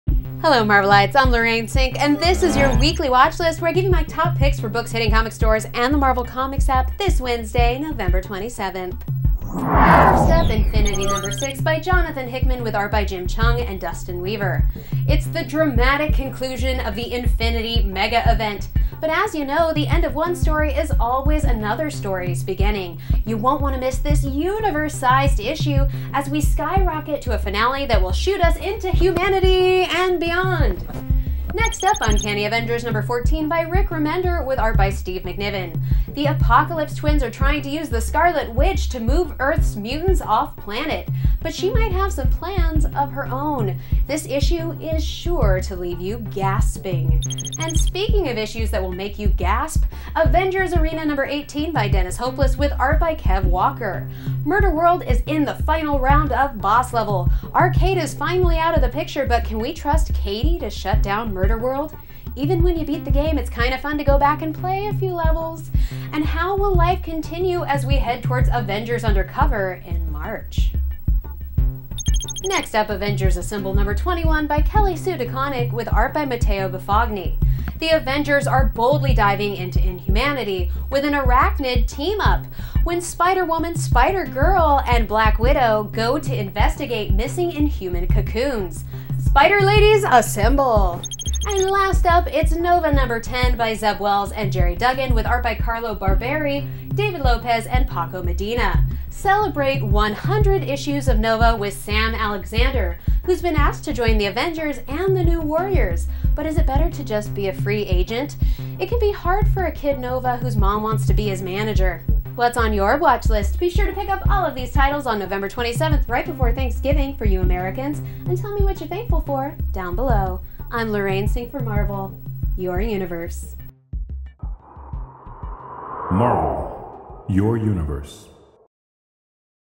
Nova #100 - The Watch List 11-27-13
Look out for the thrilling conclusion of cosmic drama coming in Infinity #6 (Hickman, Chueng, Weaver) , X-treme outcomes in Uncanny Avengers #14 (Remender, McNiven), poor sportsmanship in the dramatic end of Avengers Arena #18 (Hopeless, Walker), the assembly of an all spider-lady team-up in Avengers Assemble #21 (DeConnick, Buffagni), and celebrate 100 issues of Nova with Nova #10 (Wells, Duggan, Barberi, Lopez, Medina)! It's a big week of comics you're sure to feel thankful for!